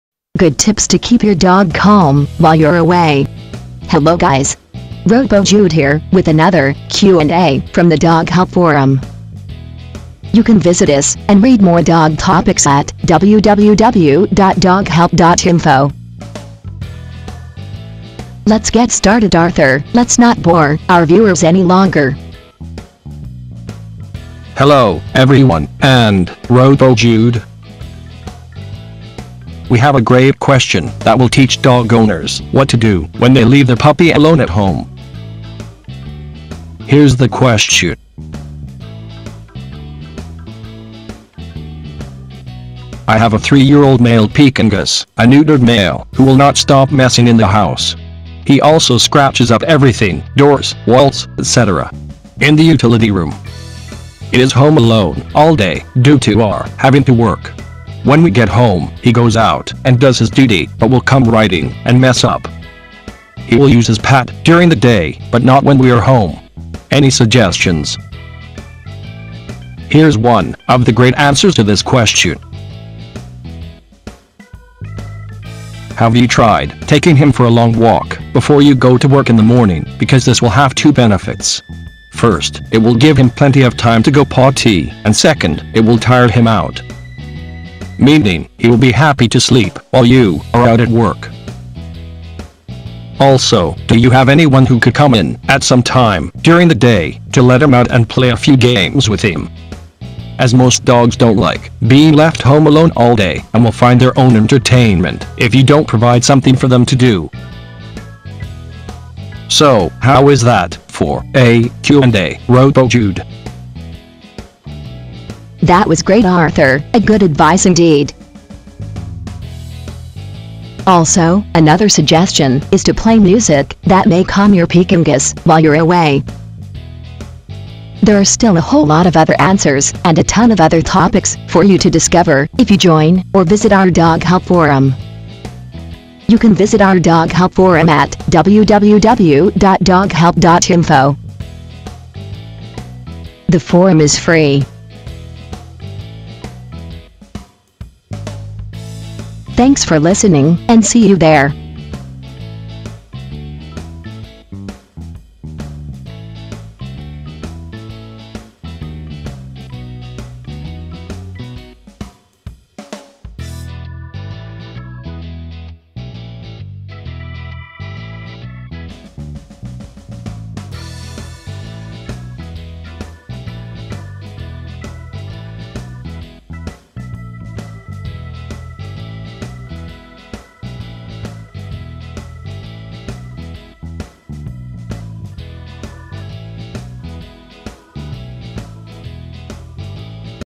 Good Tips To Keep Your Dog Calm While You Are Away
We have a great question that will teach dog owners what to do when they leave their puppy alone at home.